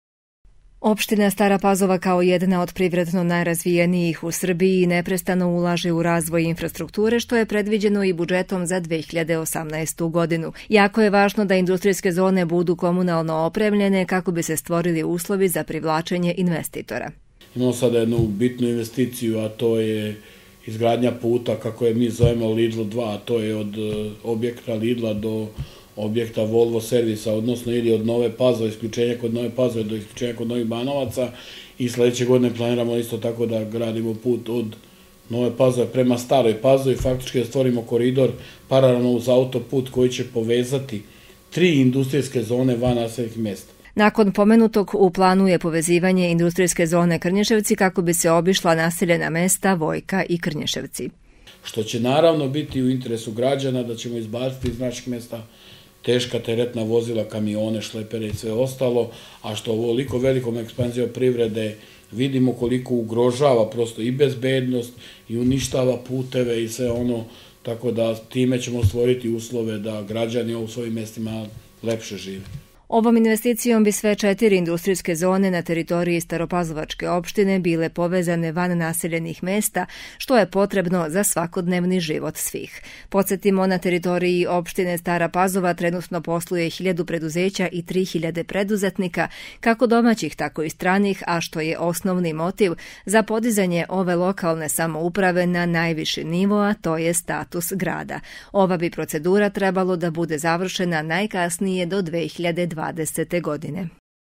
Povezivanje industrijskih zona
Dobra komunalna opremljenost  predstavlja osnovu za razvoj privrede i privlačenje investitora. Upravo u tom cilju, rukovodstvo lokalne samouprave u Staroj Pazovi radi na povezivanju industrijskih zona sa glavnim saobraćajnicama i autoputem, ali takođe i na povezivanju  4 industrijske zone van naseljenih mesta, kako bi se smanjile saobraćajne gužve i stvorili uslovi da građani lepše žive u svojim mestima.